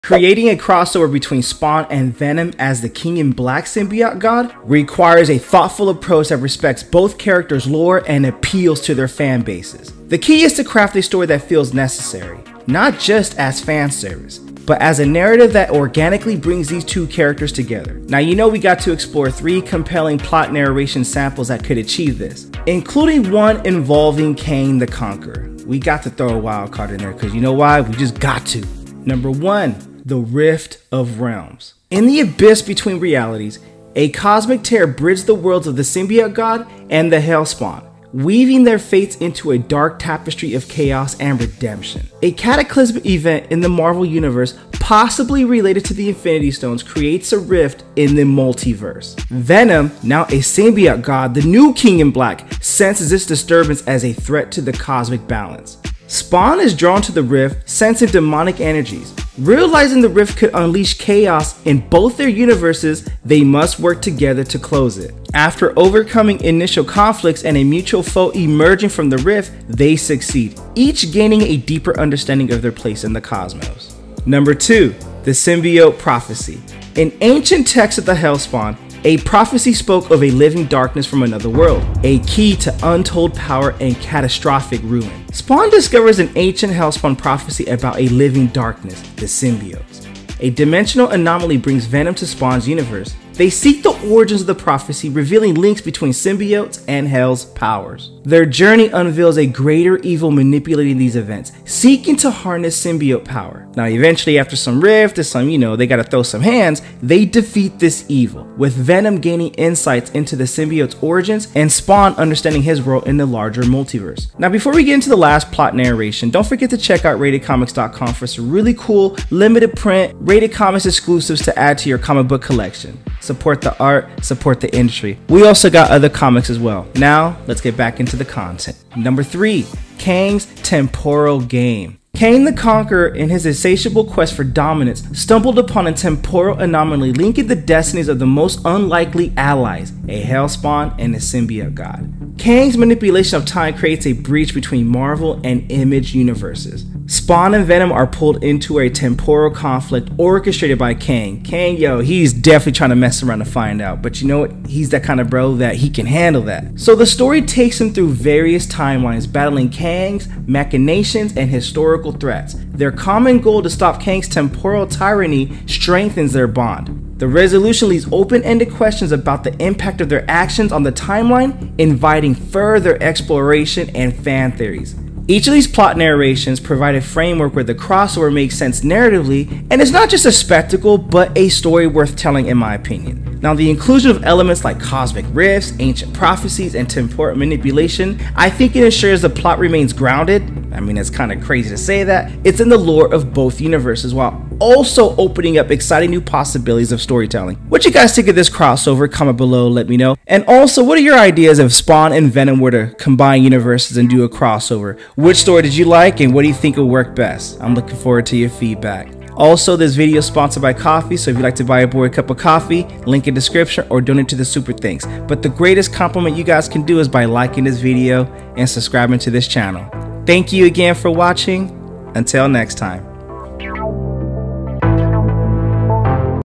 Spawn Vs Venom Symbiote God | Darkness & Fury Collide
Spawn Vs Venom the Symbiote God. Darkness and Fury Collide

** Two titans clash in an epic battle for supremacy! **

** Venom, the King in Black Symbiote God, faces off against Spawn, the Hellspawn warrior! **

** Will Spawn's dark powers be enough to defeat Venom's alien might? **

** Or will Kang the Conqueror tip the scales in his own twisted game? **

** Don't miss this epic crossover that fans have been waiting for! **

Equipment I Use To Make This Video:

Filming: iPhone

Amazon Venom
Amazon Kang the Conqueror